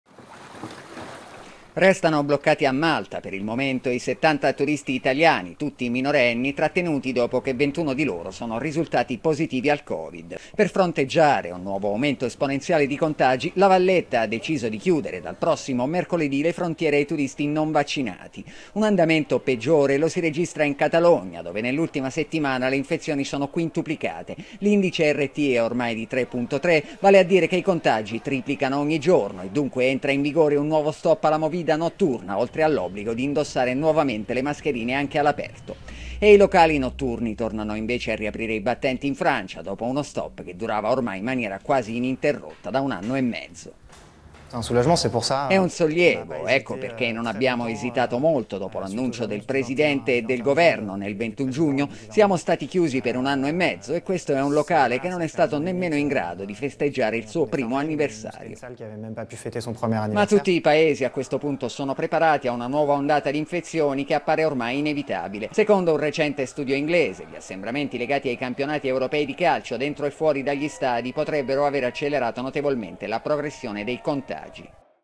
Covid, l'Europa al bivio: Malta chiude ai non vaccinati, nuovo blocco in Spagna
Settanta turisti minorenni sono rimasti bloccati a Malta, quando 20 di loro sono risultati positivi: la paura dei genitori è che non ottengano le terapie adeguate